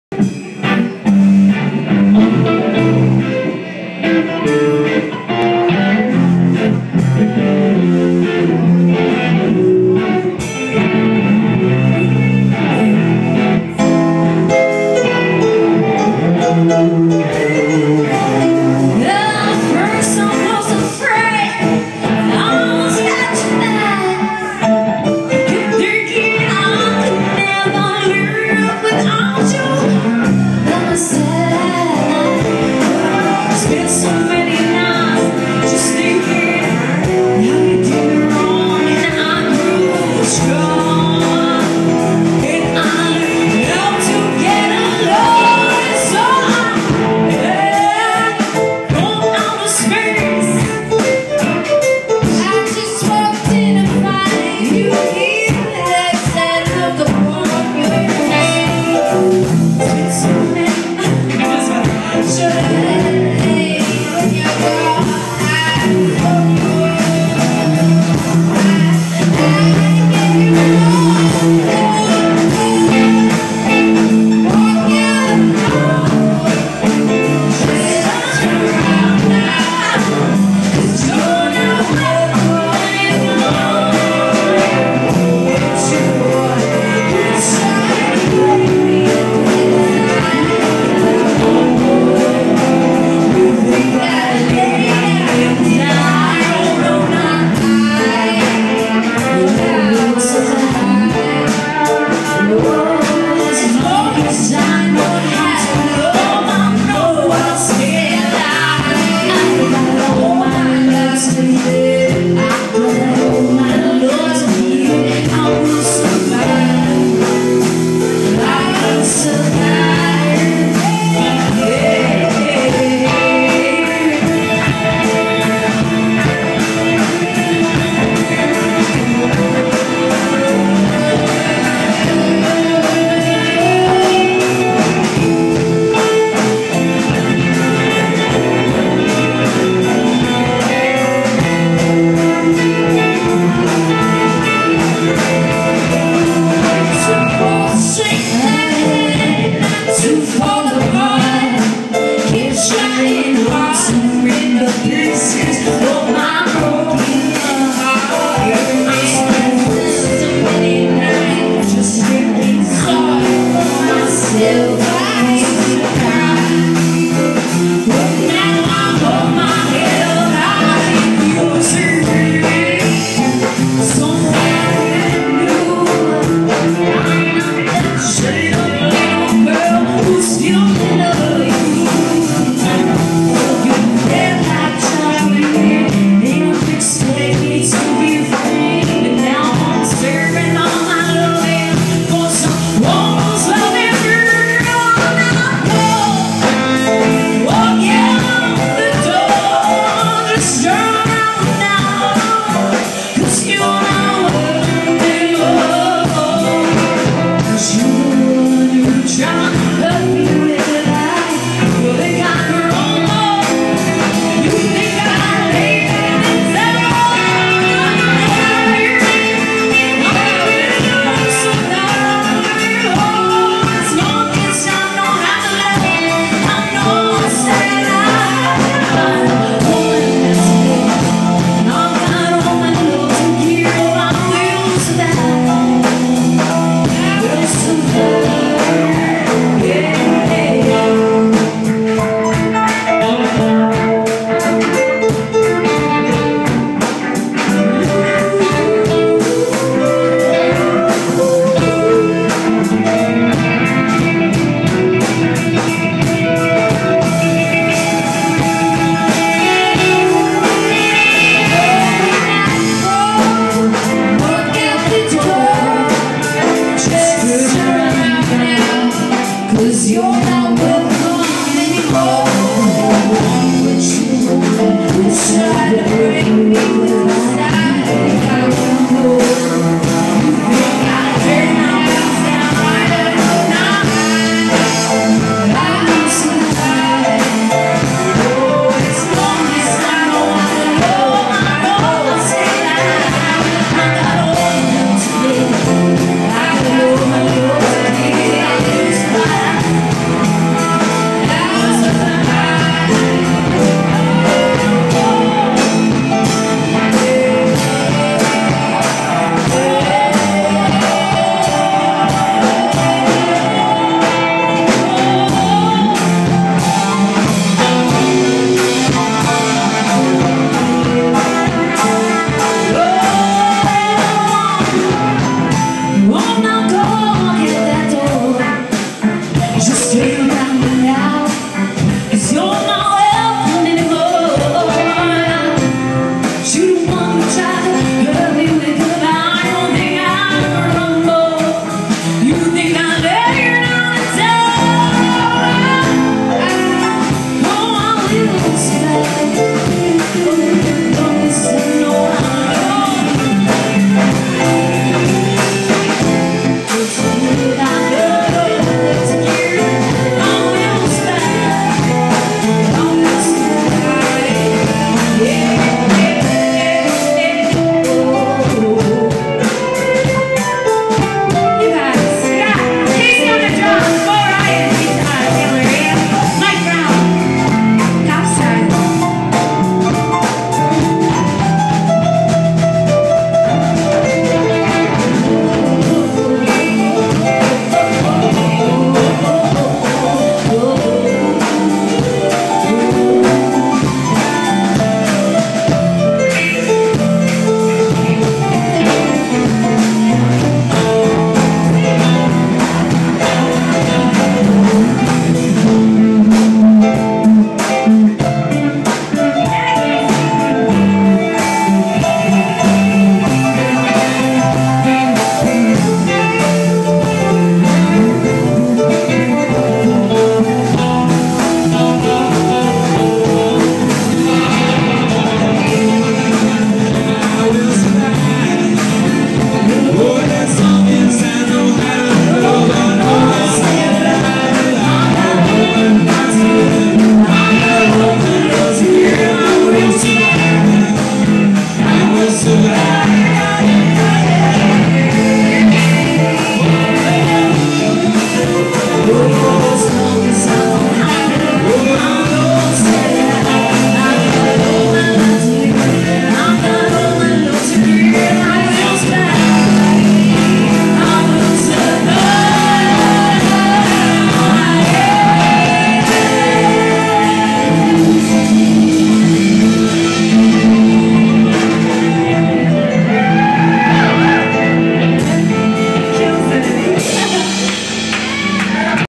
Amanda and friends
Amanda Ashley 29th Birthday Extravaganza
The Firehouse Saloon
Rochester, NY